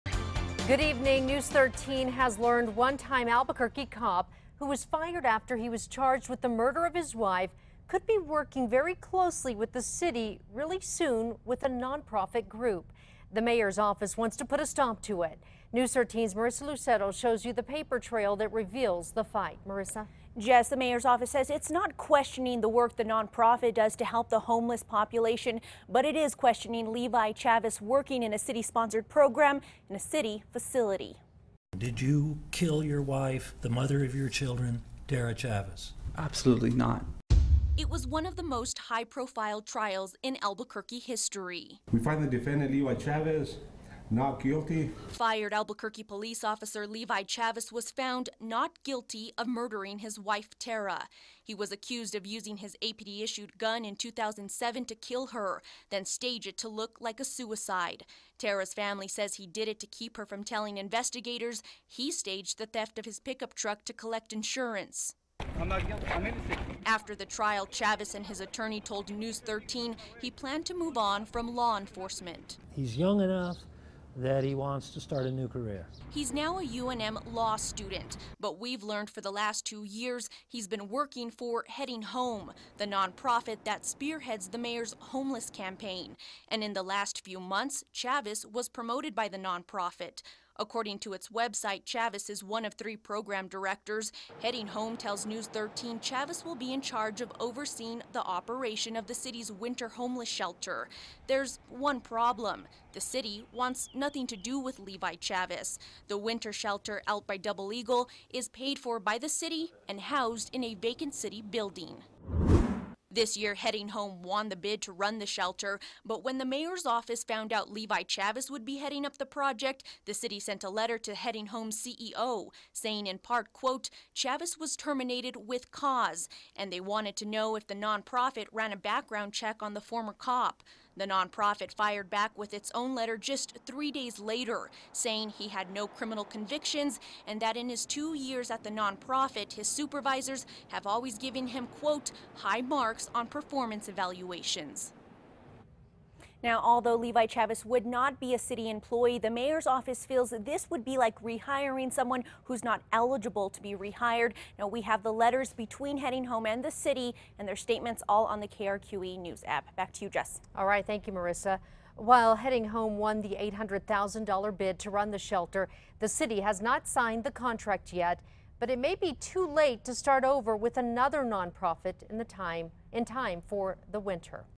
City questions prominent non-profit’s hire of ex-cop who was accused of murde
KRQE News 13 has learned former Albuquerque Police officer Levi Chavez, who was fired after he was charged with the murder of his wife, could soon be working closely with the city as part of a non-profit group.